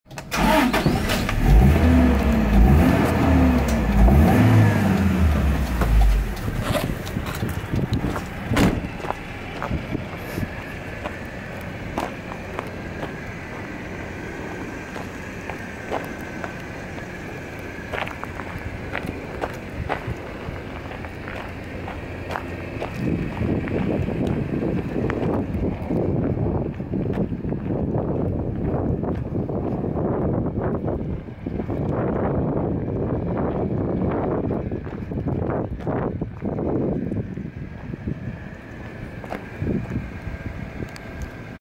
Japanese Used Cars!1997 Toyota Hiace Van RZH112V (Ref:SBP02006) We are Carused.jp!!
Ref no SBP02006
Model code RZH112V
Deck High Deck
Registration Year/Month 1997/12
Mileage 128,721km
Transmission MT
Fuel Petrol
Engine size 2,000 cc
Engine code unknown
Steerings Right
Drivetrain 2WD
Doors 4
Seats 9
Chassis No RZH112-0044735
Dimension (LxWxH) 4.69 x 1.69 x 1.96m